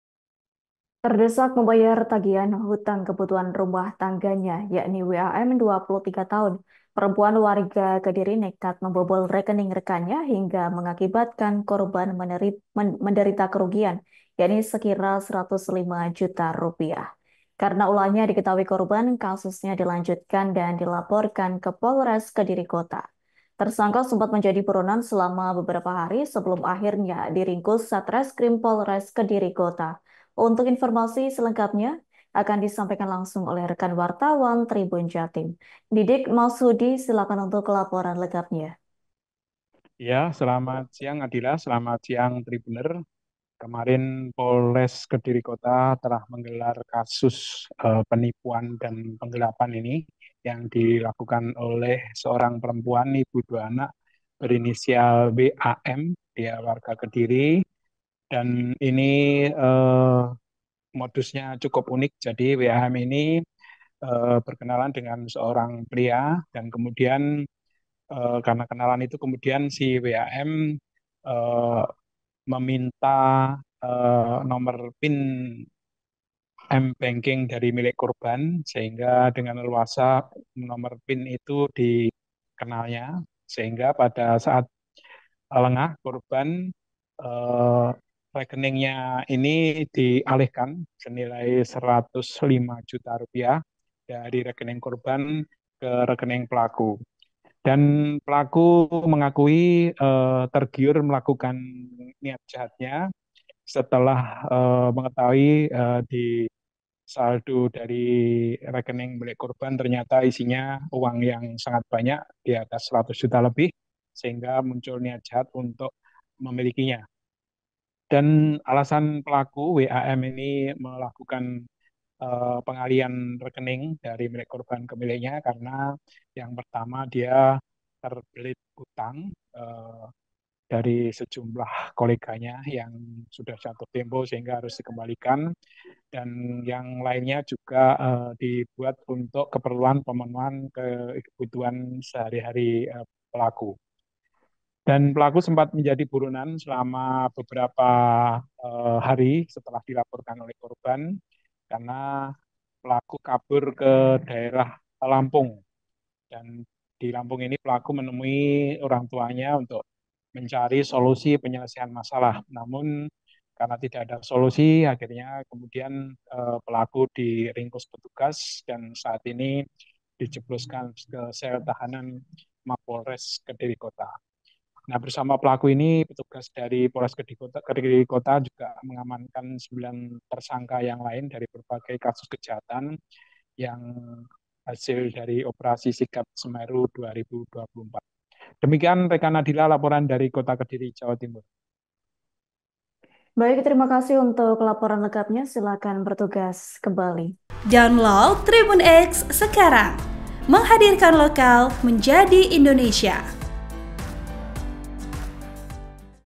Kelabakan Tak Bisa Bayar Utang, Wanita di Kediri Nekat Bobol Rekening Teman Senilai Rp 105 Juta
TRIBUN-VIDEO.COM- Terdesak membayar tagihan hutang  kebutuhan rumah tangganya, WAM (23) perempuan warga Kediri nekat membobol rekening rekannya hingga mengakibatkan korban menderita kerugian Rp 105 juta.

Karena ulahnya diketahui korban, kasusnya selanjutkan dilaporkan ke Polres Kediri Kota.

Tersangka sempat menjadi buronan selama beberapa hari sebelum akhirnya diringkus Satreskrim Polres Kediri Kota.

Program: Live Update
Host: Adila Ulfa Muna Risna
Editor Video: Latif Ghufron Aula
Uploader: Restu Riyawan